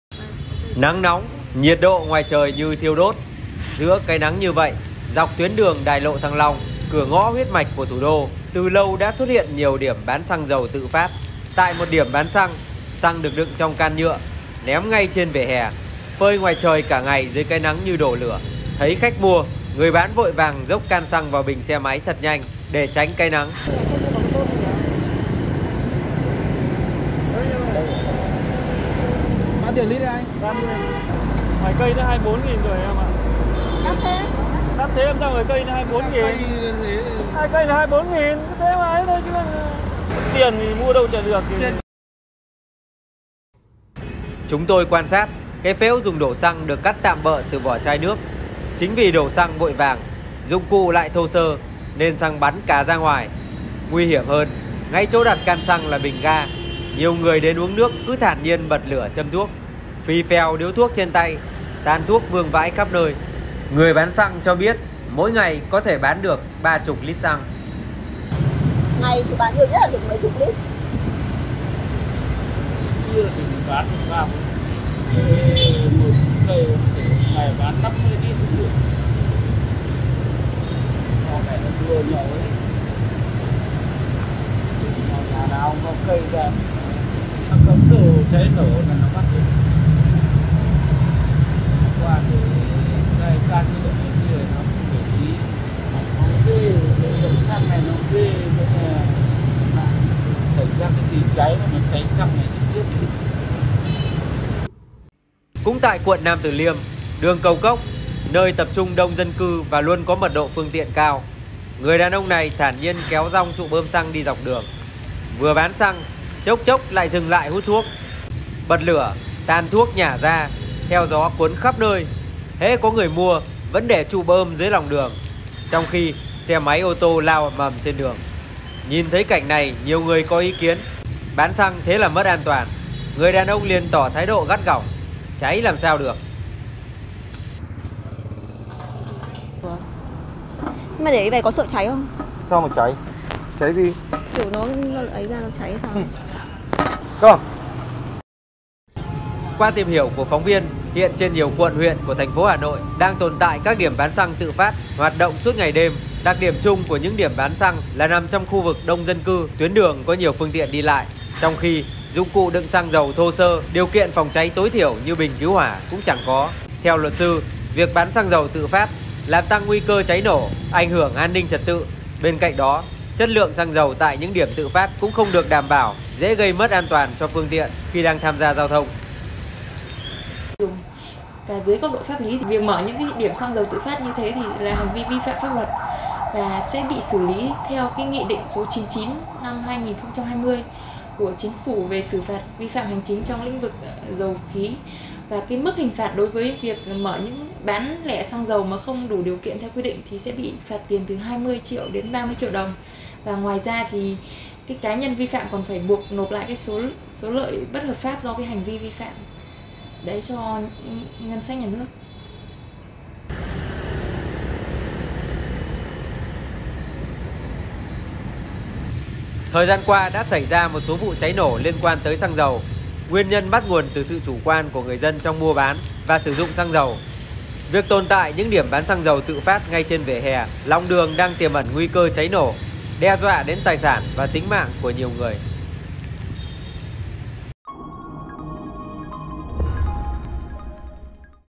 Hiểm họa cháy nổ từ những điểm bán xăng dầu tự phát trên vỉa hè, lòng đường | DV
Báo điện tử Dân Việt | Xăng dầu được đựng tạm bợ trong vỏ chai nước. Xăng dầu được bày ngay trên vỉa hè, dưới lòng đường tấp nập phương tiện trong khu dân cư đông đúc. Những điểm bán xăng dầu tự phát như vậy tiềm ẩn nhiều nguy cơ mất an toàn cháy nổ.